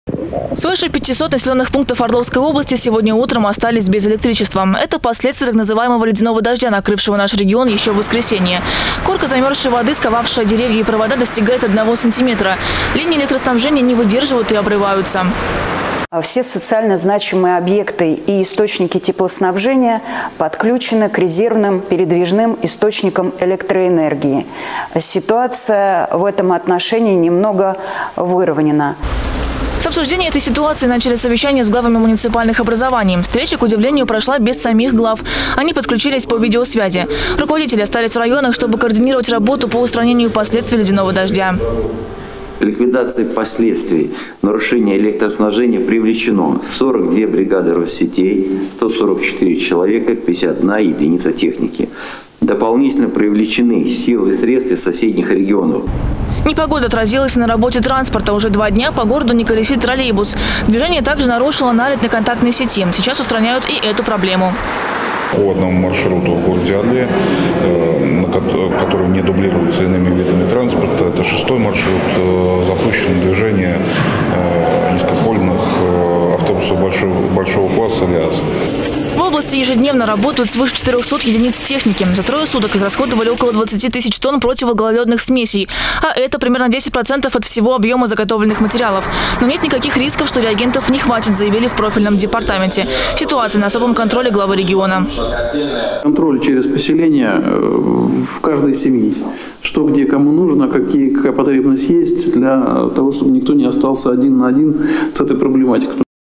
На Орловщине устраняют последствия ледяного дождя.
Свыше 500 населённых пунктов Орловской области сегодня утром остались без электричества.
Это последствия ледяного дождя, атаковавшего наш регион в течение нескольких дней, начиная с воскресенья.
Корка замёрзшей воды, сковавшая деревья и провода, местами достигла 1 сантиметра. Линии электроснабжения не выдерживают и обрываются.